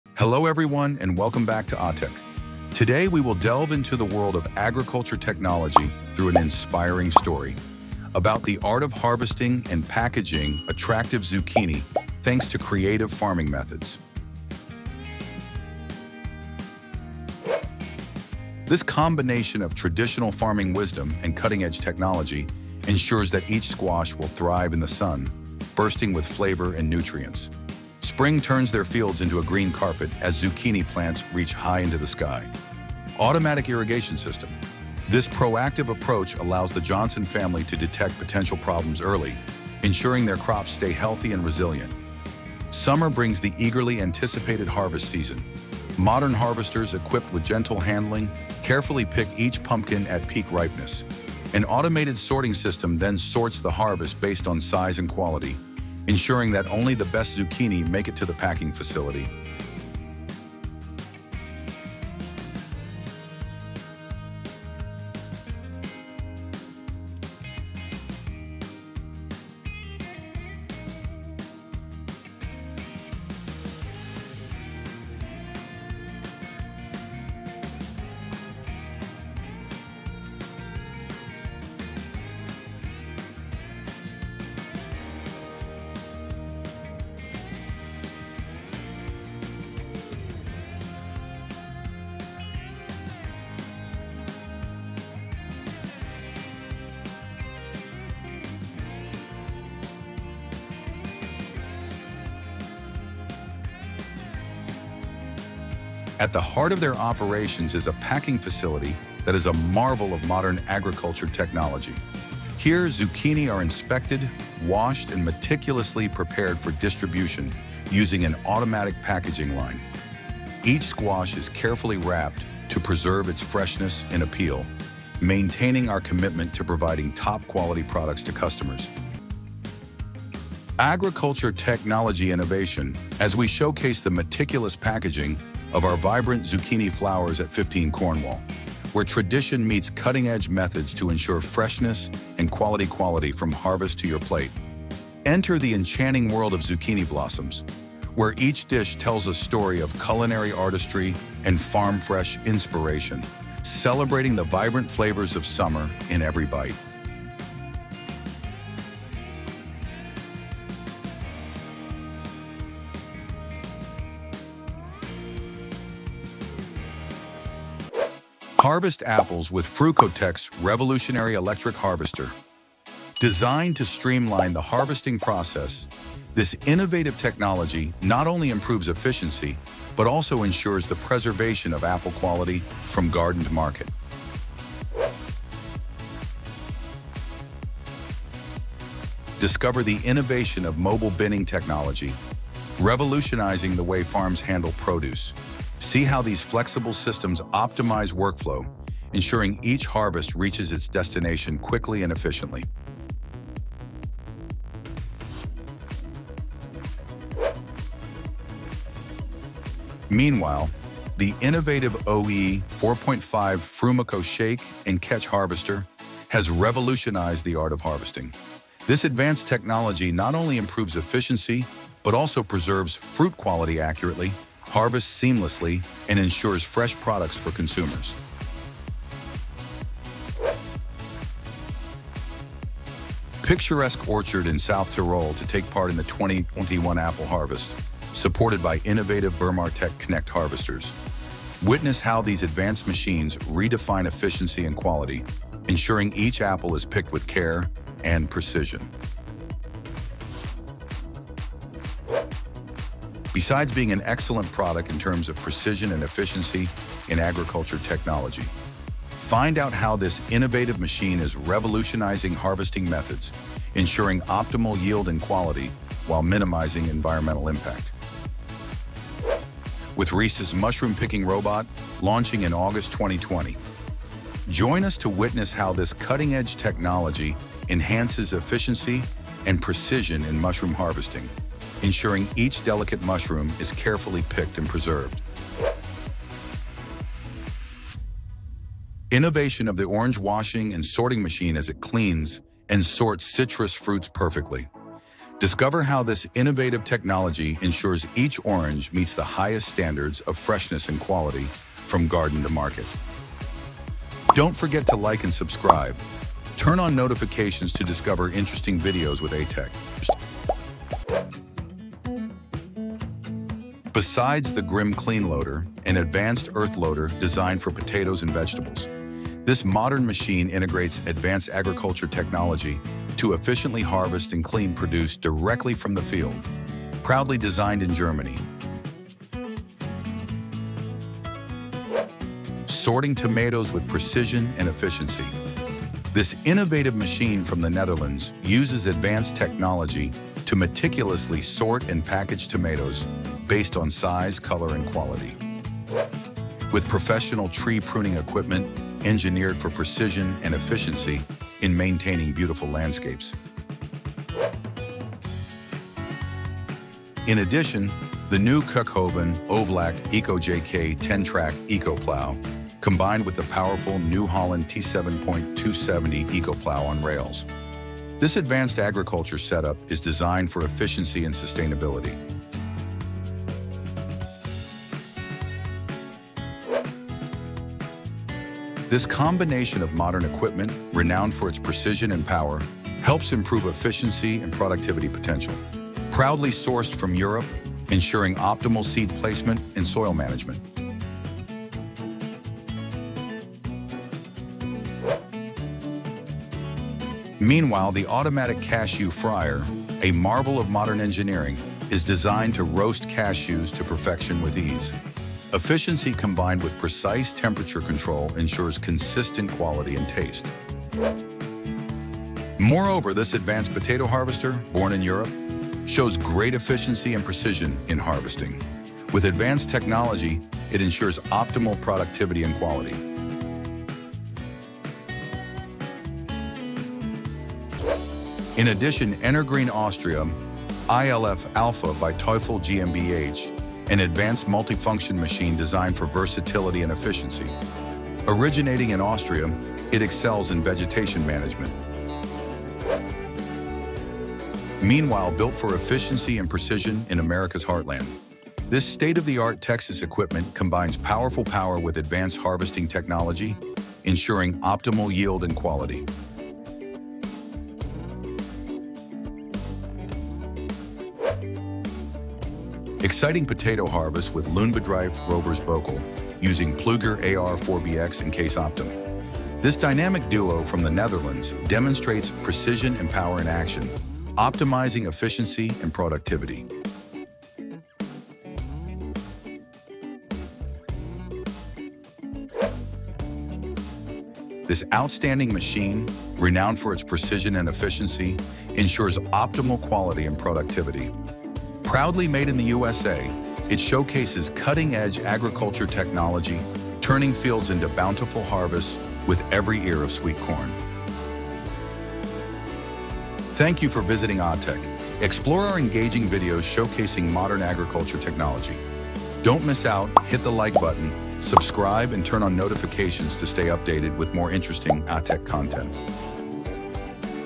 40 Most Satisfying Agriculture Technology ► 24 | How To Harvest And Process Zucchini Attractively
40 Most Satisfying Agricultural Technologies ► 24 | How To Harvest And Process Zucchini Attractively

Welcome to AhTech! Today, delve into the art of harvesting and packaging attractive zucchini using advanced agriculture technology. With automated irrigation systems in spring and modern harvesters in summer, only the finest zucchinis are carefully picked and sorted. At their state-of-the-art packing facility a marvel of agriculture technology zucchinis undergo meticulous inspection, washing, and automated packaging to preserve freshness and quality. Join us at Fifteen Cornwall, where culinary excellence meets farm-fresh inspiration in every zucchini dish, celebrating the vibrant flavors of summer. Experience the harmony of tradition and innovation in agriculture technology as we showcase the meticulous process from harvest to your plate.

We will comply with your request as soon as we receive your message.

00:00 Intro
00:18 Harvest attractive zucchini
02:48 Harvesting apples
04:29 Mushroom picking robot
05:54 Earthmoving machine
08:02 Corn harvester

AhTech